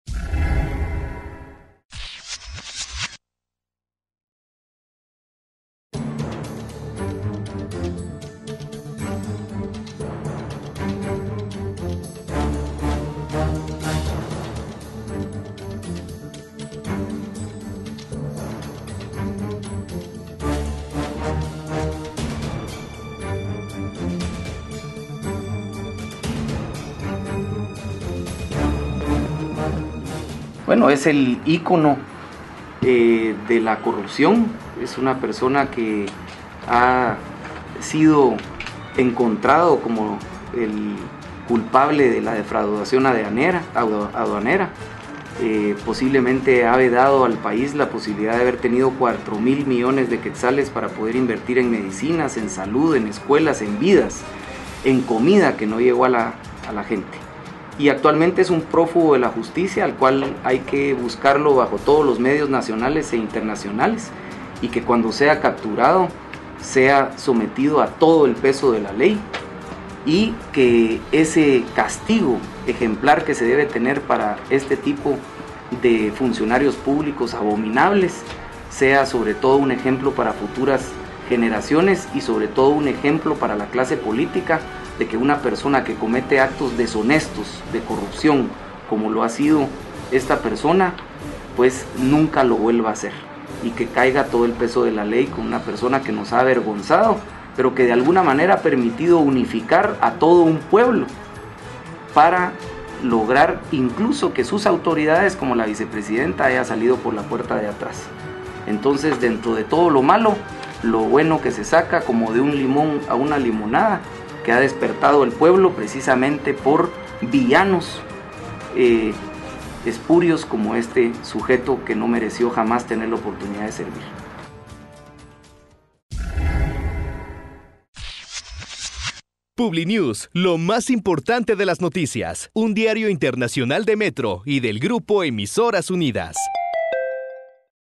Monólogo: Roberto González “Canela” arremete contra el exsecretario de Baldetti
Monólogo Publinews es un segmento en el que se les presenta a candidatos a diversos puestos de elección popular 5 tarjetas con temas de coyuntura nacional, eligen uno y en un minuto deben desarrollarlo dejando clara su postura sobre el tema. En esta ocasión, el turno fue para el candidato a la presidencia por el partido Compromiso, Renovación y Orden; y el Partido Unionista (Creo-PU), Roberto González